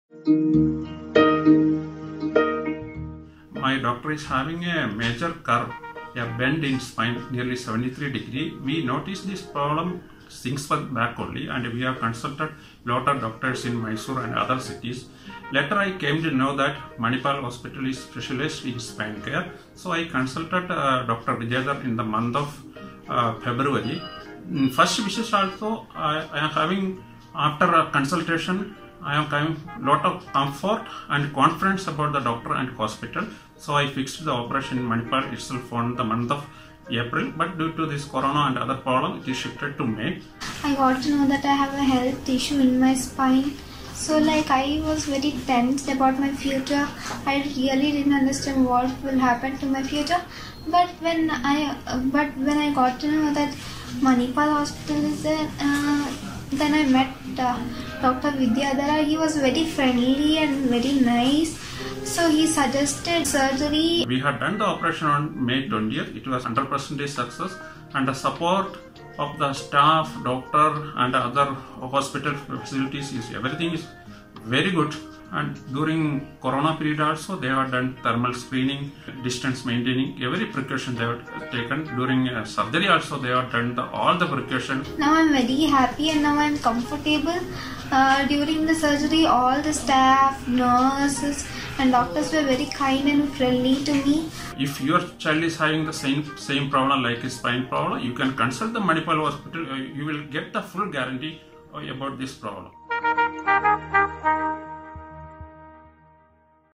Ms. Gopika | Dr. S Vidyadhara | Spine Surgery | Manipal Hospitals India
When Ms. Gopika’s spine showed an extreme curve of more than 73 degrees, her parents visited Manipal Hospitals Old Airport Road, where under the expert guidance of  Dr. S Vidyadhara, HOD and Consultant - Spine Surgery, Manipal Spine Care Center, she underwent surgery keeping all the sanitary and safety measures at the highest order of priority and is now on a smooth road to recovery. Watch here!